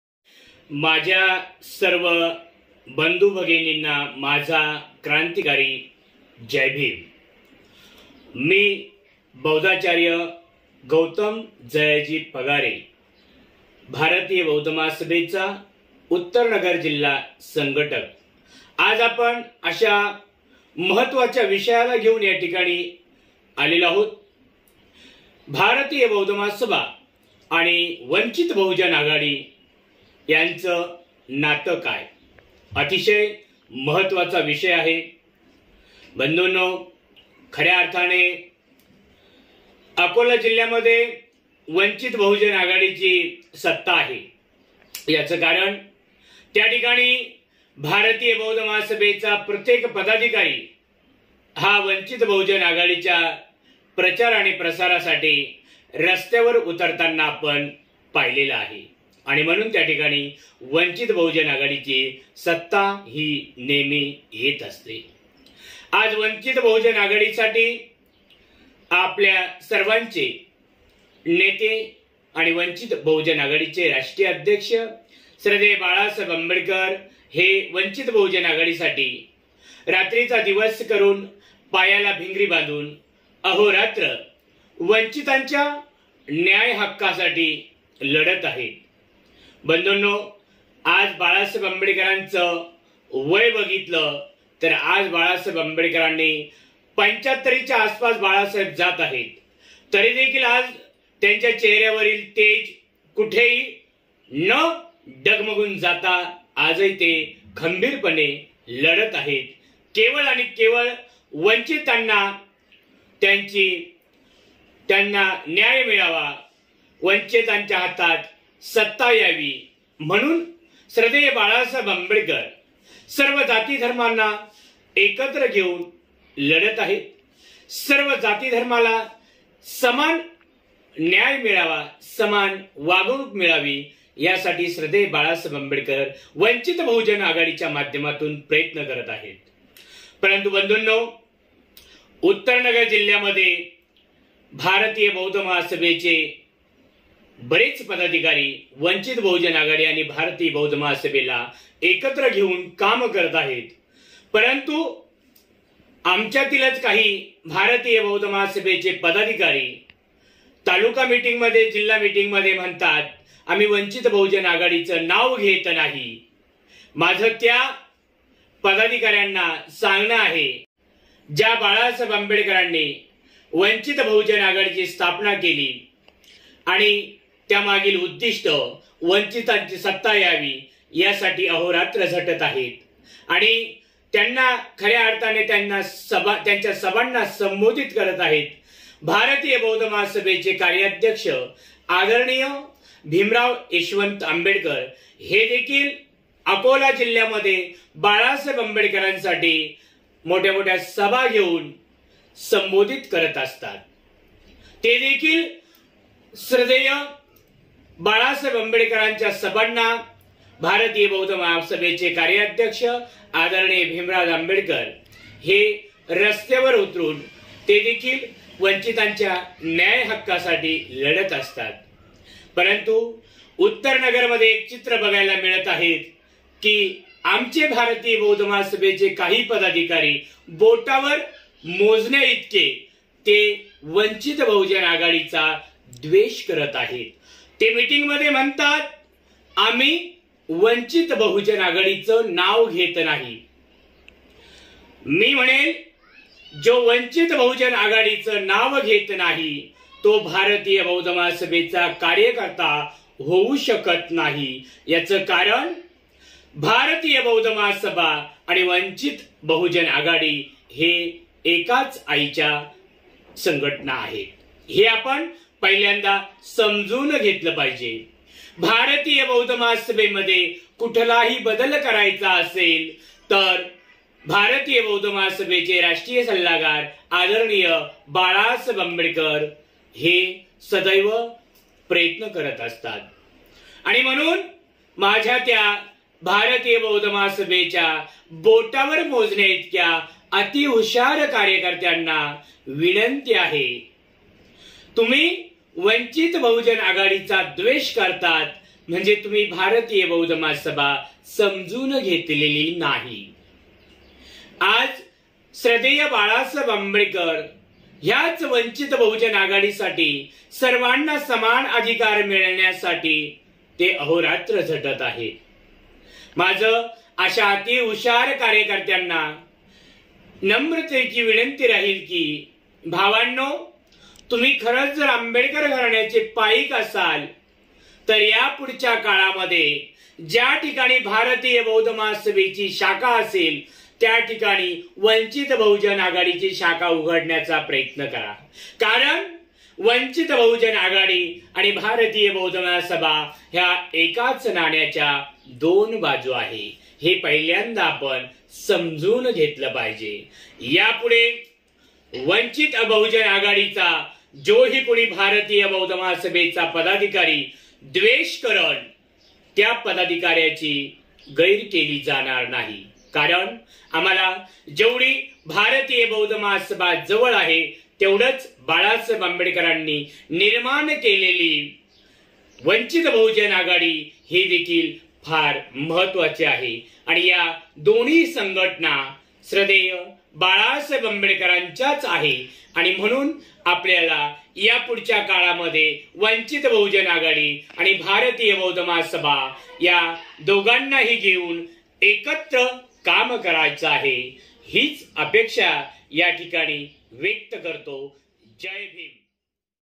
भारतीय बौद्ध महासभा आणि वंचित बहुजन आघाडी यांच नात काय?|गौतम पगारे|Vba
भारतीय बौद्ध महासभा* *आणि वंचित बहुजन आघाडी याच नातं काय?
   वंचित बहुजन आघाडी निर्माण करतांना श्रध्देय बाळासाहेब आंबेडकर यांनी खुप मेहनत आणि कष्ट घेतलेले आहेत.रात्रीचा दिवस आणि रक्ताचे पाणी करून साहेब अहोरात्र झटत आहेत.वंचित घटकांला न्याय देण्याच काम *श्रध्देय बाळासाहेब आंबेडकर करीत आहेत...
   अकोला जिल्ह्यामध्ये वंचित बहुजन आघाडीची सत्ता आहे,यांचे कारण तिथे *भारतीय बौद्ध महासभेचा प्रत्येक पदाधिकारी,हा वंचित बहुजन आघाडीसाठी काम करतो.
  स्वत: भारतीय बौद्ध महासभेचे कार्याध्यक्ष आद भिमराव यशवंत आंबेडकर हे देखील तिथल्या प्रचार सभांना संबोधित करतात.वंचित बहुजन आघाडीच्या प्रचाराला पैसा कमी पडू नये म्हणूनस्वत:च्या गळ्यातील मंगळसूत्र देताना आपण महिलांना बघितले आहे.
   अहमदनगर जिल्ह्यातील बरेच पदाधिकारी हे भारतीय बौद्ध महासभेचे, आणि वंचित बहुजन आघाडीचे काम अतिशय प्रामाणिक पणे करीत आहे.
         परंतु भारतीय बौद्ध महासभेचे काही पदाधिकारी (बोटावर मोजता येणारे)हे बोलता आम्ही वंचित बहुजन आघाडीचे  नाव घेत नाही... त्यांच्या बुद्धीची किव कराविशी वाटते,त्यांना सांगावं वाटतं,तुमच्यात वंचित बहुजन आघाडी बदल एवढा राग(द्वेष)का?
              वंचित बहुजन आघाडी आणि भारतीय बौद्ध महासभा, ह्या दोन्हीही संघटना एकाच आईच्या आहे...एकाच नाण्याच्या दोन बाजू आहेत,हे आपण समजून घेतले पाहिजे, भारतीय बौद्ध महासभेमध्ये जर काही बदल करायचा असेल तर,उदा.फळे,पाणे, धागा ,पाणी सोडणे हे सर्व प्रकार श्रध्देय बाळासाहेब आंबेडकर यांनी बंद केले आहे.
वंचित बहुजन आघाडीचा तिरस्कार करणाऱ्यांनो, वंचित बहुजन आघाडी साठी साहेब पायाला भिंगरी बांधून रात्रं दिवस अहोरात्र मेहनत घेत आहेत.साहेबांना कधी कधी पुढील सभा ऐटेन्ट करण्यासाठी साहेबांना गाडीतच बसून जेवण करावं लागतं आणि आमच्यातील काही हुशार म्हणतात आम्ही वंचित बहुजन आघाडीचे नाव घेत नाही...हे सर्व लिहिण्याचे कारण.
    ज्यांना वंचित बहुजन आघाडीचा तिरस्कार आहे अशा अतिहुशार लोकांसाठी माझे नम्र आवाहन आहे !
           अरे भावांनो,भारतीय बौद्ध महासभा आणि वंचित बहुजन आघाडी ह्या दोन्हीही संघटना श्रध्देय बाळासाहेब आंबेडकर यांच्या आहेत हे विसरतात कामा नये.ज्याला वंचित बहुजन आघाडीचे नाव घ्यायला लाज वाटते तो भारतीय बौद्ध महासभेचा पदाधिकारी कधीच होवु शकत नाही...
          कारण श्रध्देय बाळासाहेब आंबेडकर यांच्या प्रचारासाठी भारतीय बौद्ध महासभेचे कार्याध्यक्ष आद.भिमराव यशवंत आंबेडकर हे स्वत: जातीने रस्त्यावर उतरतात...कित्येक सभा  घेतात...मग आमच्यातील काही अतिहुशारांना वंचित बहुजन आघाडी बदल एवढा तिरस्कार का...?
      आज श्रध्देय ॲड.बाळासाहेब आंबेडकर यांचे वय बघा, त्यांचा समाजासाठीचा त्याग...कष्ट बघा..! आणि त्यांच्या चेहर्‍यावरील तेज बघा!
ऊन असो..वारा असो..पाऊस असो साहेब कधीच डगमगत नाही.विचार करा..त्यांना हे सारं करायची काय गरज आहे?त्यांनी ही इतर आंबेडकरी म्हणवून घेणा-या नेत्यासारखा आपल्या आणि आपल्याच कुटुंबियांचा स्वार्थ आणि सत्तेची हाव पाहिली असती तर ते आणि त्यांचे कुटुंबीय आजन्म सत्तेत असते आणि पैशांच्या राशीवर ऐशोआरामात झोपले असते..!
    आपले उध्दारक विश्वरत्न बोधिसत्व  डाॅ.बाबासाहेब आंबेडकर यांचे रक्ताचेच नव्हे तर विचार आणि आचार यांचे सुध्दा खरे वारसदार म्हणून आपण आंबेडकर कुटुंबाप्रति एकनिष्ठ आणि प्रामाणिक राहून या सामाजिक, राजकीय आणि धार्मिक संघर्षात थन मन धनाने सक्रीय राहणे हे आपले सर्वांचे आद्य कर्तव्य आहे कदापिही विसरता कामा नये! नव्हे तेच आपले जीवनाचे प्रथम आणि अंतिम उद्दिष्ट असले पाहिजे!
             माझ्या भावांनो, वंचित बहुजन आघाडी स्थापन करताना साहेबांनी वंचितांना न्याय कसा मिळेल... वंचित सत्तेत कसा येईल...मग तो कुठल्याही जातीधर्माचा असो!प्रत्येकाला समान अधिकार देण्याचे काम श्रध्देय ॲड.बाळासाहेब आंबेडकर करत आहेत.
          अकोला जिल्ह्यामध्ये जसे वंचित बहुजन आघाडी आणि भारतीय बौद्ध महासभा ह्या एक होऊन काम करतात,तेच काम उत्तर नगर जिल्ह्यात देखिल व्हायला हवं.
    आपण जर खरोखर आंबेडकर घराण्याचे पाईक असाल तर, यापुढच्या काळात आपण वंचित बहुजन आघाडी आणि भारतीय बौद्ध महासभा ह्या दोन्हीही संघटना एकाच नाण्याच्या दोन बाजू आहेत हे ओळखून आपन काम कराल आणि  वंचित समाजासाठीच्या लढाईत श्रध्देय बाळासाहेबांना साथ  द्याल हिच प्रामाणिक आशा आनी अपेक्षा बाळगतो...धन्यवाद!
आपलाच धम्मबंधू,
  आयु.गौतम जी पगारे
जिल्हा संघटक
भारतीय बौद्ध महासभा, अहमदनगर जिल्हा (उत्तर)